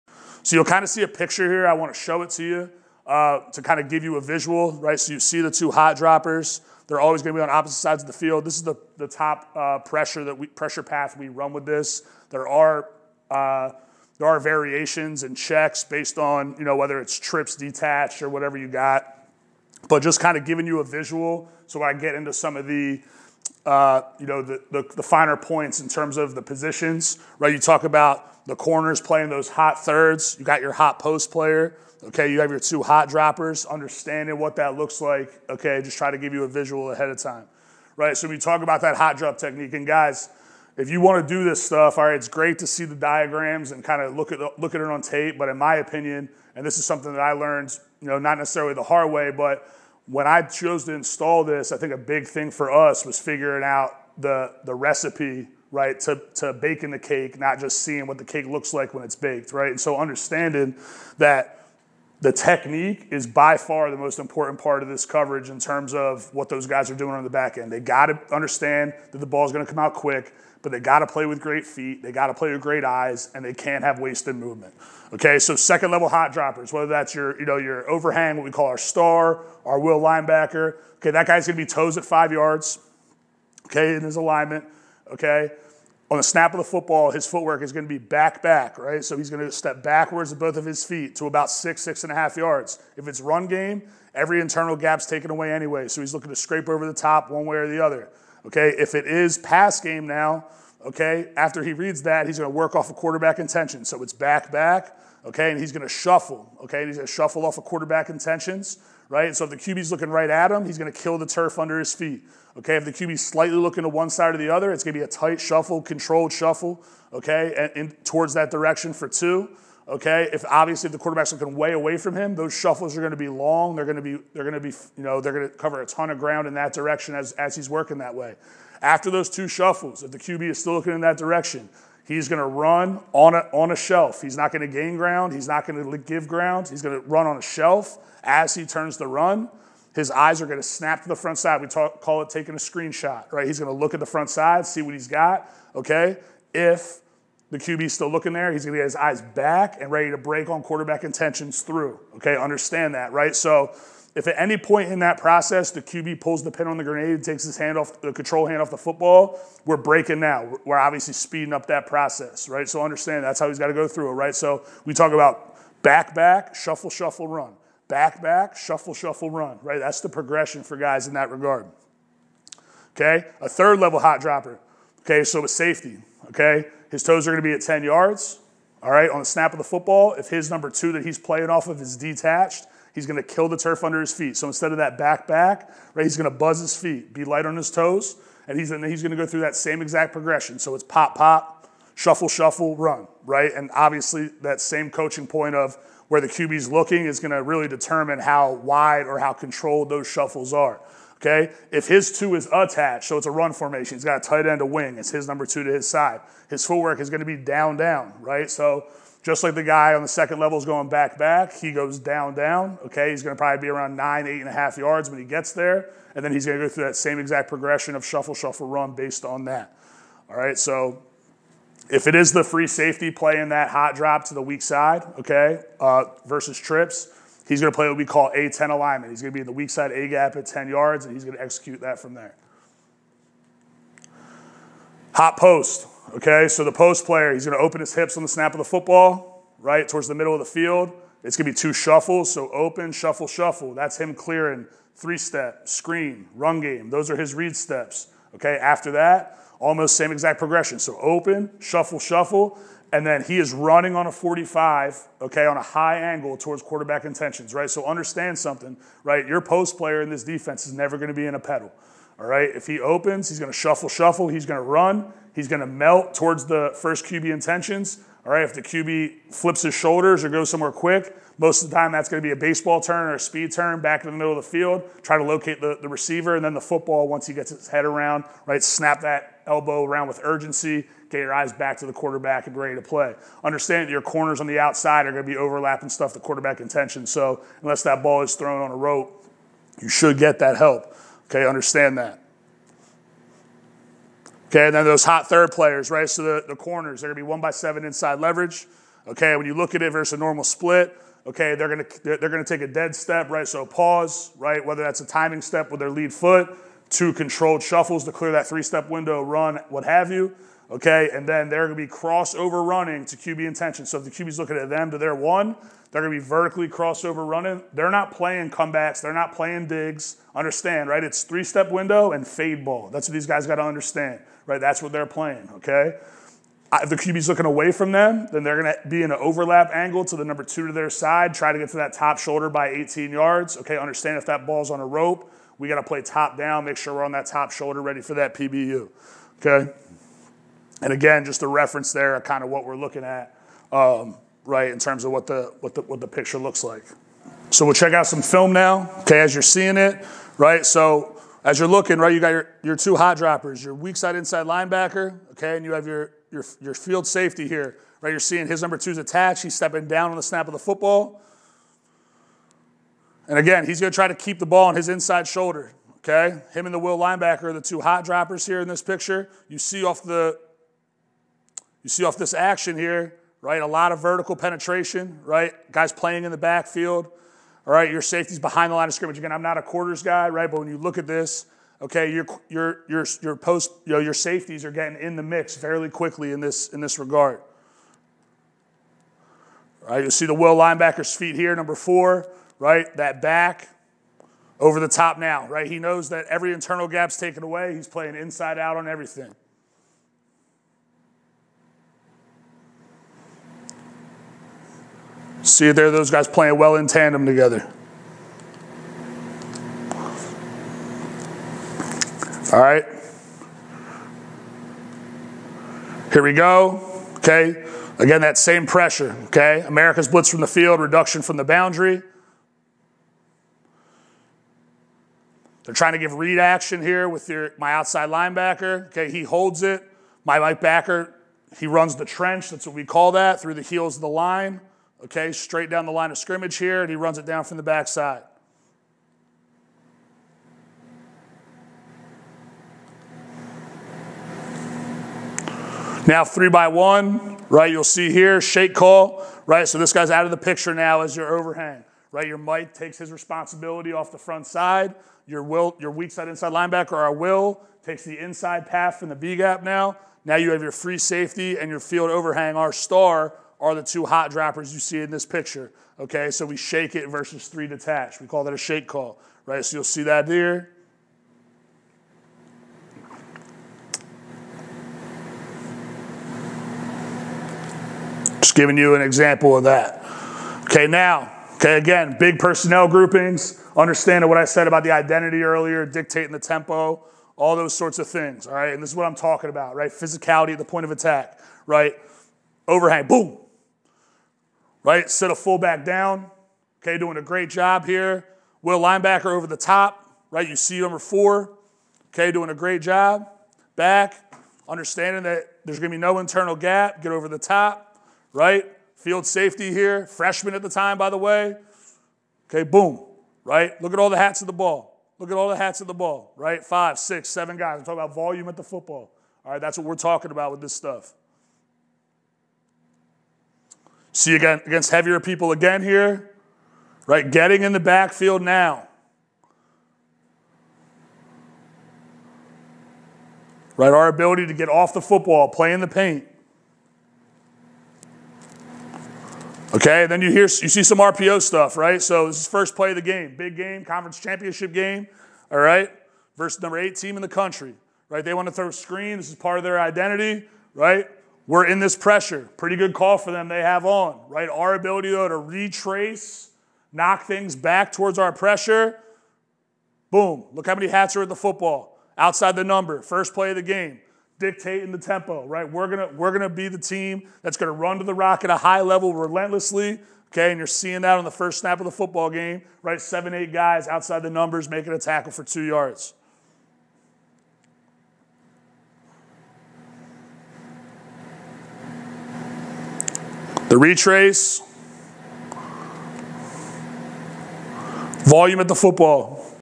Create Havoc with Pressure and Hot Drop Coverage
Pat Ruley, Head Coach Rowan University (NJ)